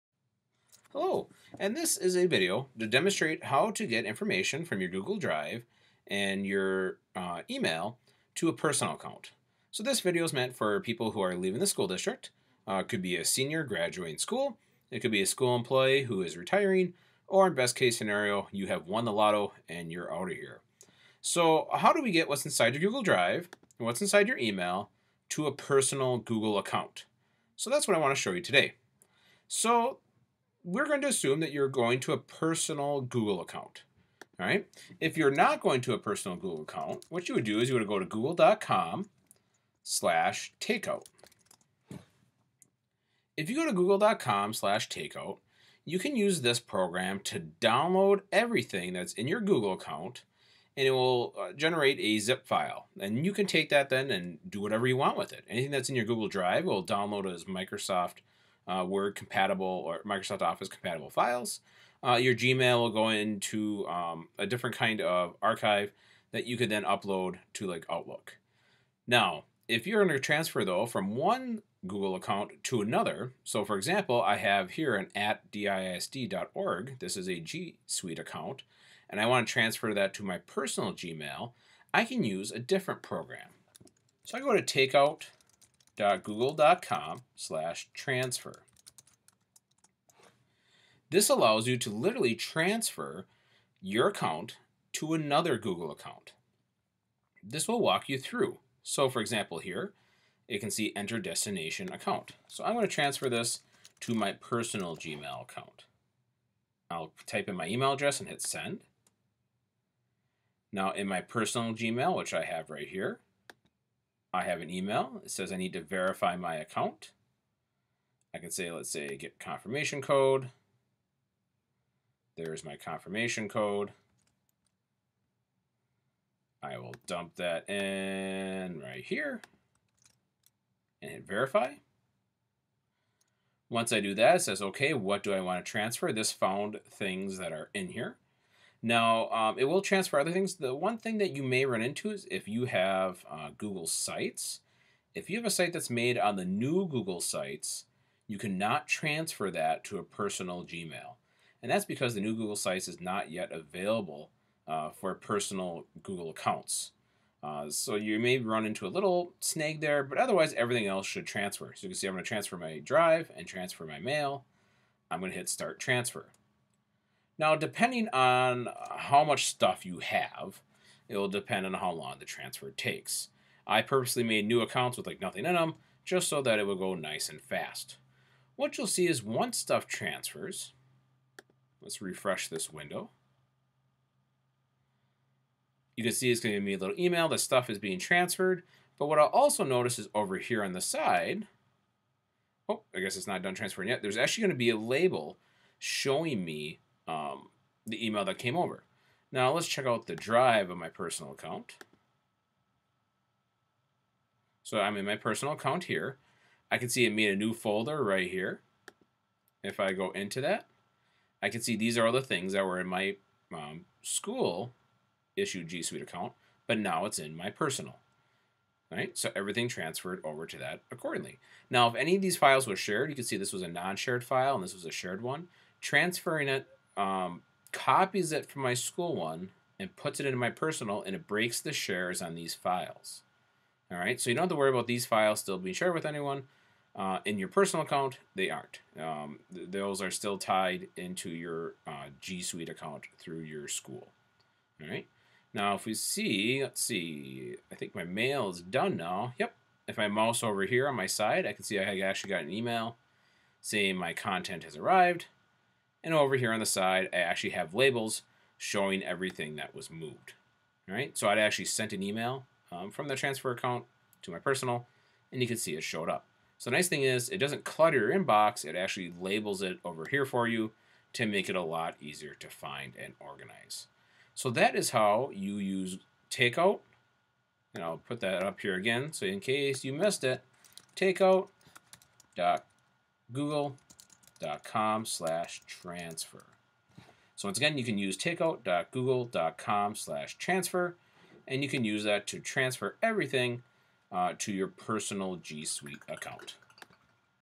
Google Takeout Transfer Tutorial
This video is a demo on how to use the new Transfer option with Google Takeout.

Edit:
The new Google Sites is now available for personal Google accounts! Thank you to fellow edtech awesome person Melinda for letting me know!